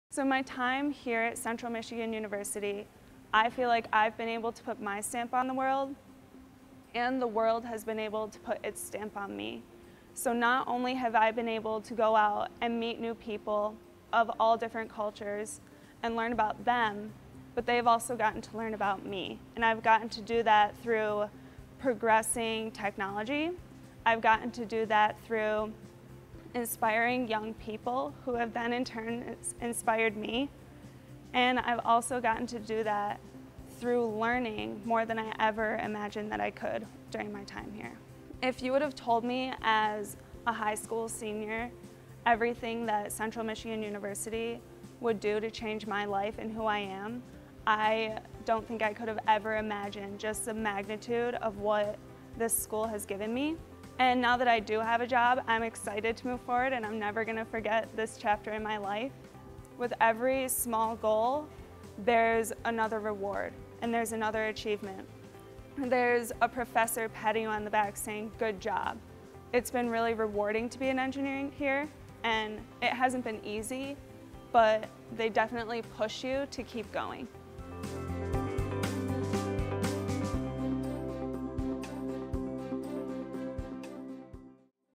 Stages of engineering success
Katherine Kolar, a Central Michigan University electrical engineering major from Brighton, has had a position waiting for her at General Motors since completing an internship there last summer. She credits her success to hands-on research experience inside the classroom and beyond, and to encouragement from faculty member Adam Mock.